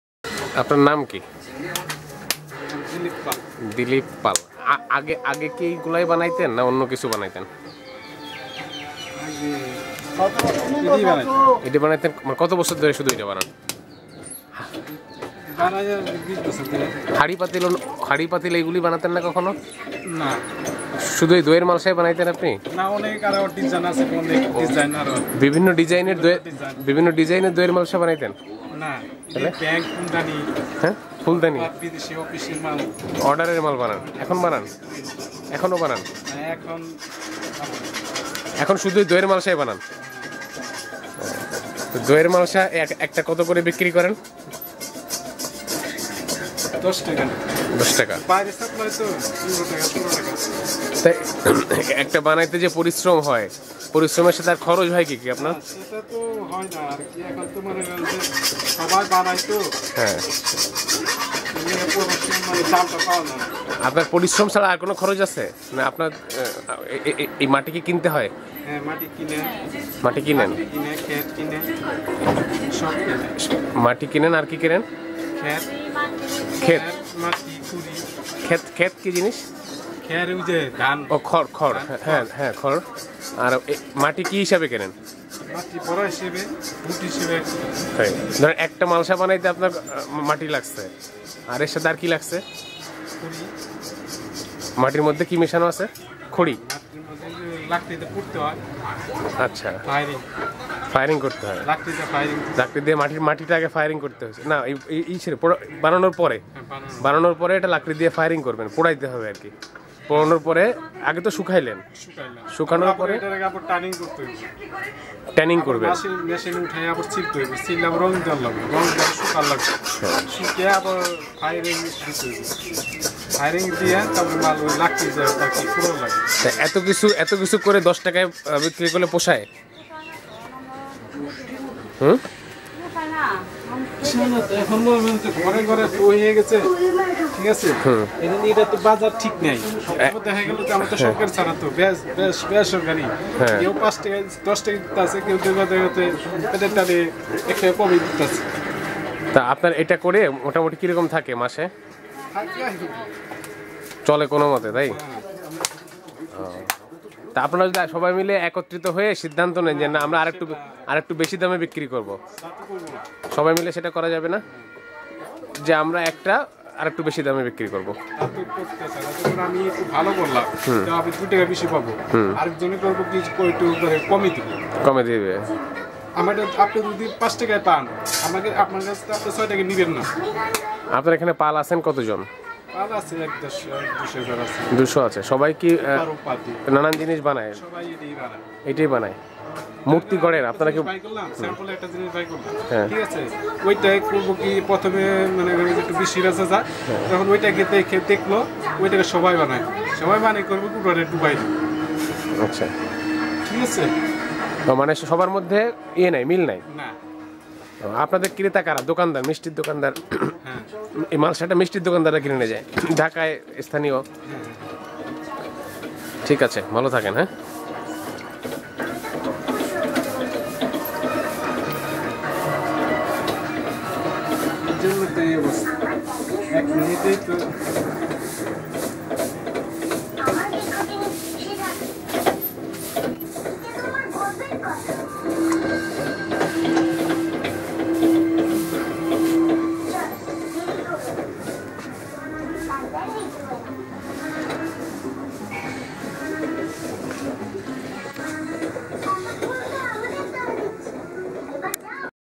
মাটি দিয়ে কুমোর যেভাবে দইয়ের মালশা বানায়, দেখেছেন কখনো?
টুকিটাকি কিছু অন্য জিনিসপত্র বানালেও নবীনগরের নলাম এবং কাকরাম গ্রামের মৃৎ শিল্পীরা প্রধানত দইয়ের মালসা বানিয়ে টিকে আছে।